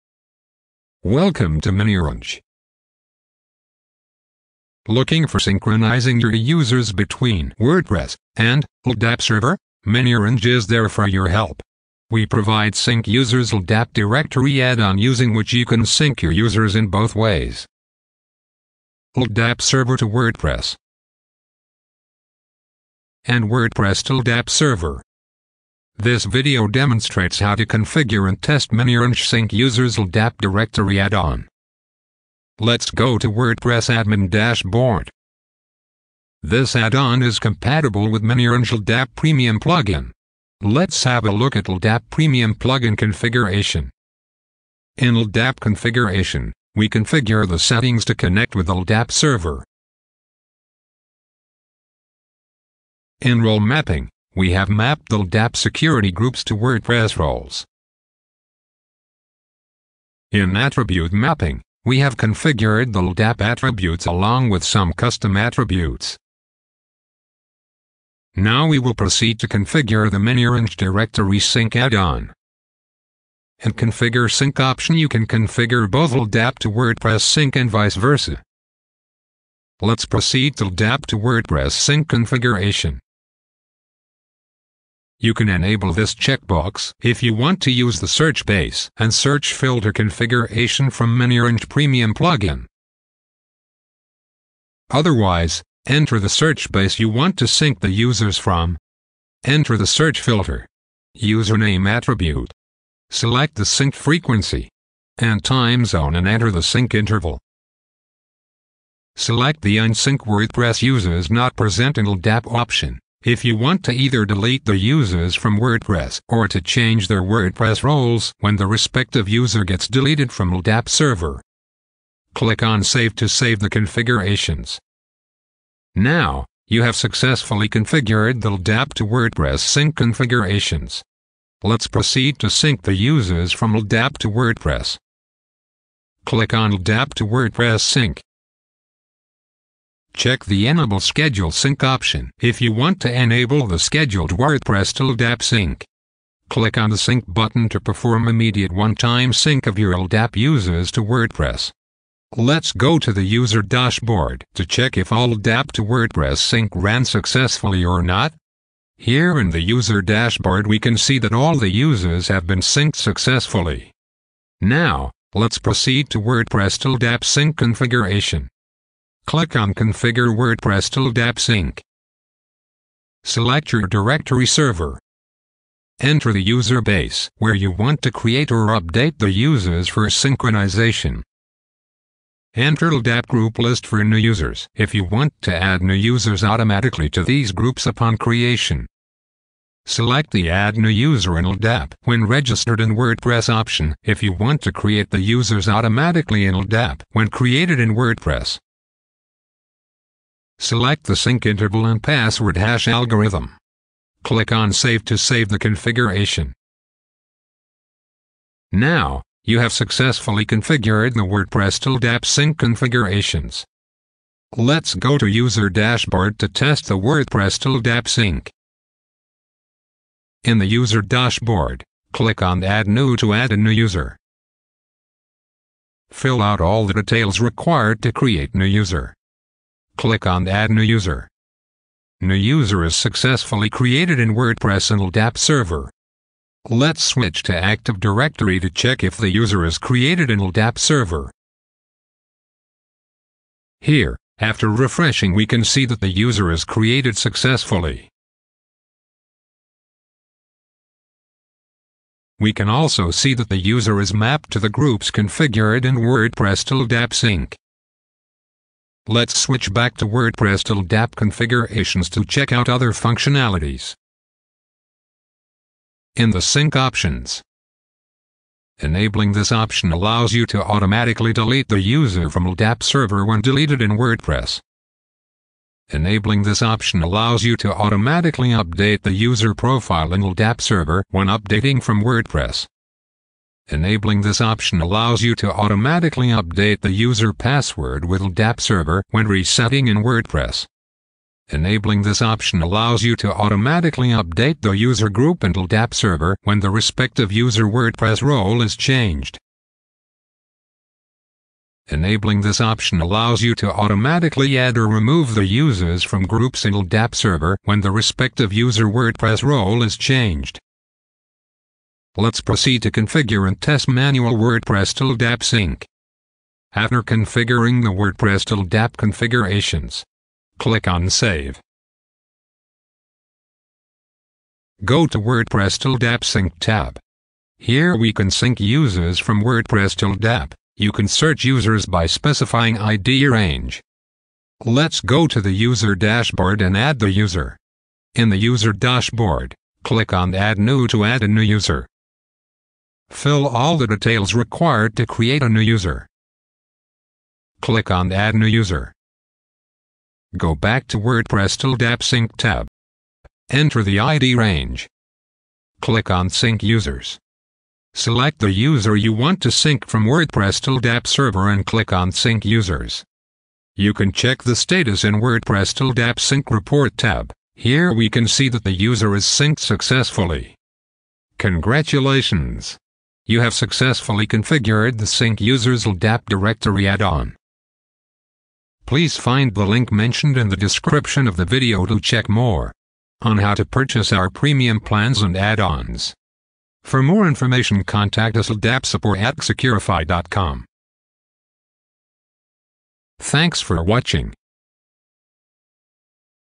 How to synchronize your users between WordPress and LDAP server? | WordPress LDAP/AD Login
The Sync WordPress users with LDAP directory add-on provides the feature to synchronize your users between WordPress and LDAP Directory.

This add-on is compatible with the LDAP/AD Login for Intranet Sites Premium Plugin.
LDAP/AD Login for Intranet sites premium plugin provides login to WordPress using credentials stored in your LDAP Server.

It allows users to authenticate against various LDAP implementations like:
1. Microsoft Active Directory
2. Azure AD
3. Sun Active Directory
4. OpenLDAP
5. JumpCloud
6. FreeIPA
7. Synology
8. OpenDS
9. and several other directory systems.

We support features like custom WordPress profile mapping, mapping LDAP groups to WordPress roles, auto-create/update LDAP users in WordPress.